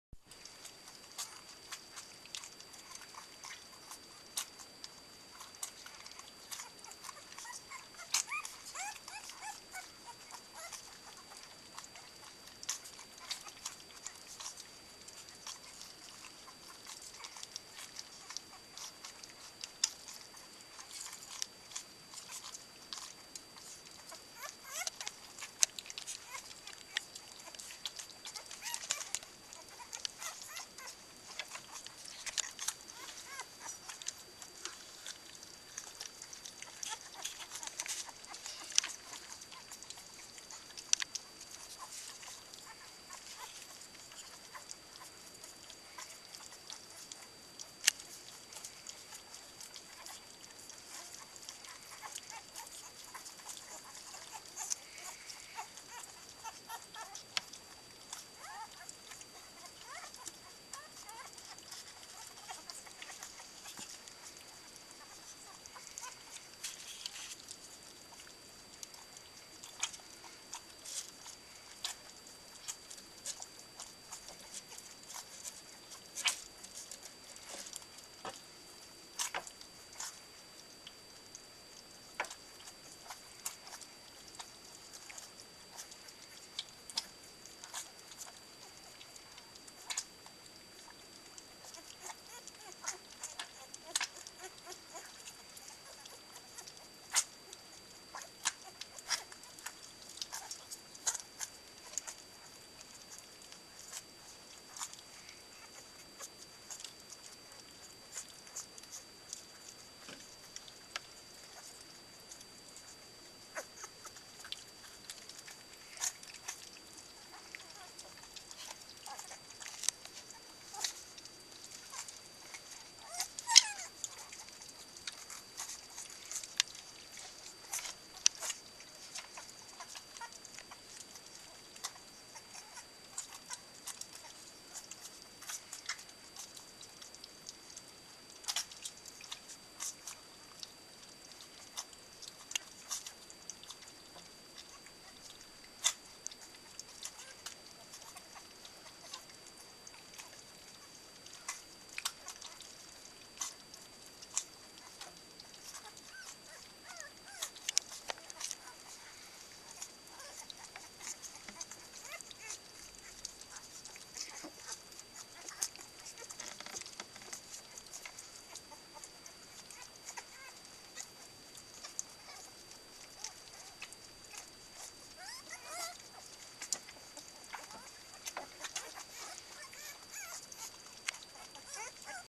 Cardigan Corgi Puppies Getting Fed
This 16 day old litter of Cardigan Welsh Corgis is getting their first ever meal from "the flying saucer", a large round bowl for feeding small puppies.